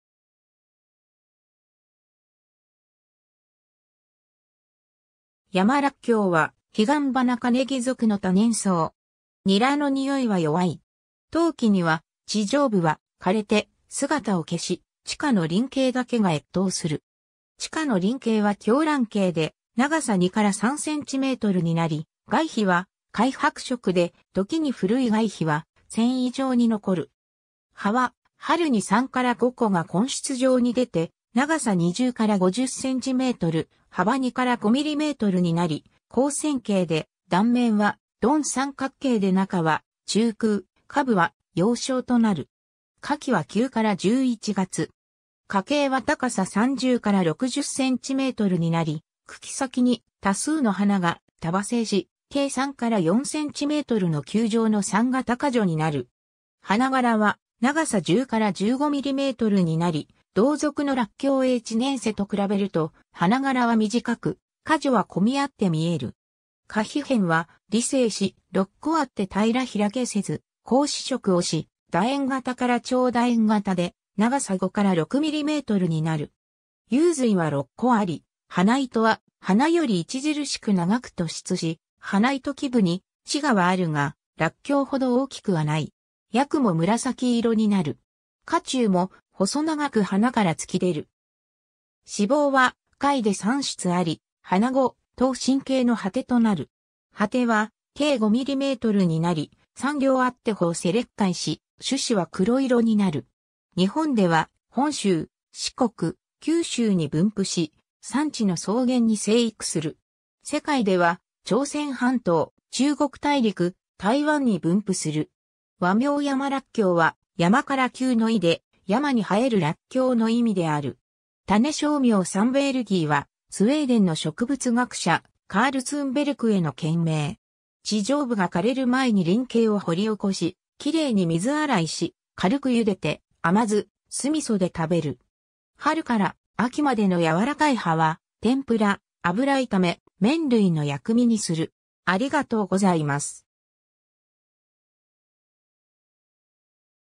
ヤマラッキョウ
ヤマラッキョウ（山辣韭、学名：Allium thunbergii）は、ヒガンバナ科ネギ属の多年草。
 ニラの臭いは弱い。
冬季には地上部は枯れて姿を消し、地下の鱗茎だけが越冬する。
 地下の鱗茎は狭卵形で、長さ2-3cmになり、外皮は灰白色で、ときに古い外皮は繊維状に残る。
葉は、春に3-5個が根出状に出て、長さ20-50cm、幅2-5mmになり、広線形で、断面は鈍三角形で中は中空、下部は葉鞘となる。
花茎は高さ30-60cmになり、茎先に多数の花が束生し、径3-4cmの球状の散形花序になる。
花柄は長さ10-15mmになり、同属のラッキョウ A. chinense と比べると花柄は短く、花序は混み合ってみえる。
花被片は離生し、6個あって平開せず、紅紫色をし、楕円形から長楕円形で長さ5-6mmになる。
雄蕊は6個あり、花糸は花より著しく長く突出し、花糸基部に歯牙はあるがラッキョウほど大きくはない。
葯も紫色になる。
花柱も細長く花から突き出る。
子房は下位で3室あり、花後、倒心形の蒴果となる。
蒴果は径5mmになり、3稜あって胞背裂開し、種子は黒色になる。
 日本では、本州（秋田県以南）、四国、九州に分布し、山地の草原に生育する。
世界では朝鮮半島、中国大陸、台湾に分布する。
 和名ヤマラッキョウは、「山辣韭」の意で、「山に生えるラッキョウ」の意味である。
 種小名（種形容語）thunbergii は、スウェーデンの植物学者、カール・ツンベルクへの献名。
 地上部が枯れる前に鱗茎を掘り起こし、きれいに水洗いし、軽くゆでて、甘酢、酢味噌で食べる。
春から秋までの軟らかい葉は、天ぷら、油炒め、麺類の薬味にする。